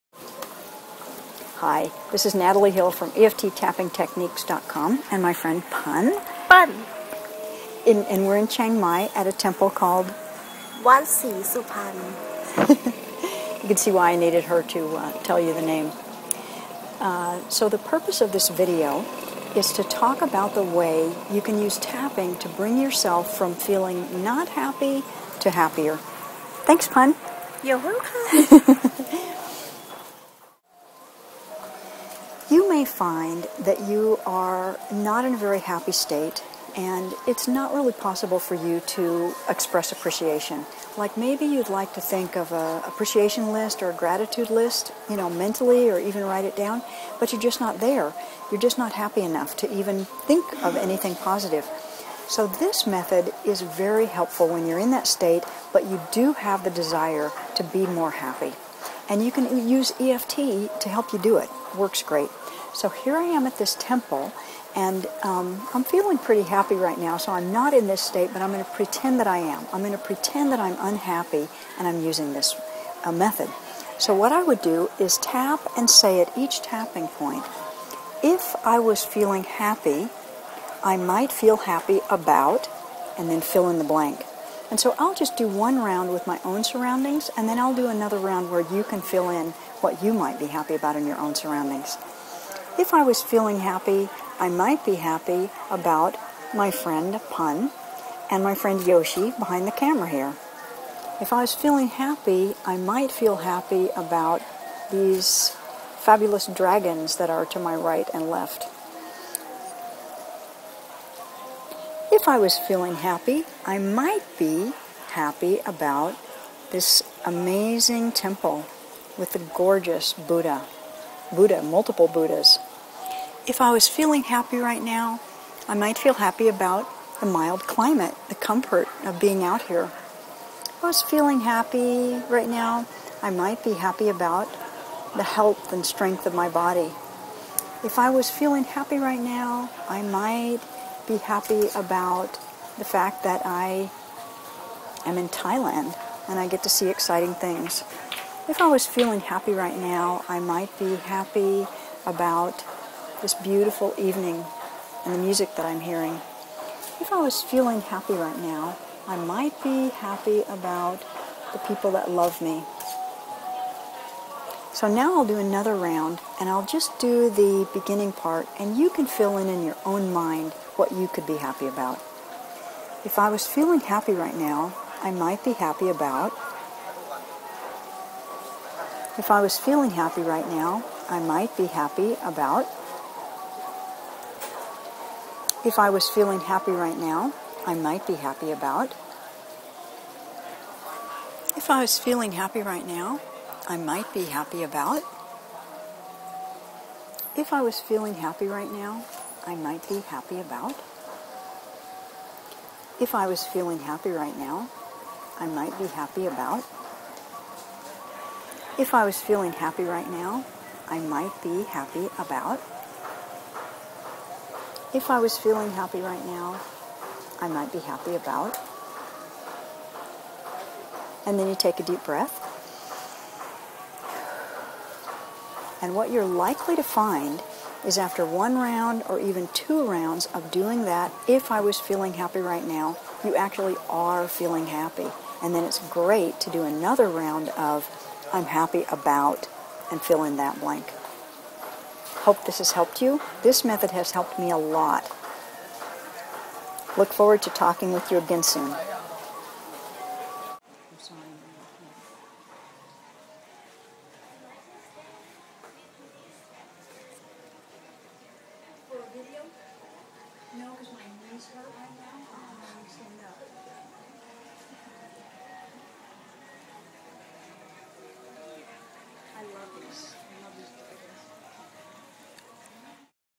EFT for Abundance - In a Thai Buddhist Temple
Learn EFT for abundance.  The happier you are, the higher your vibration.  Since like attracts like, a high, happy vibe attracts more of the same.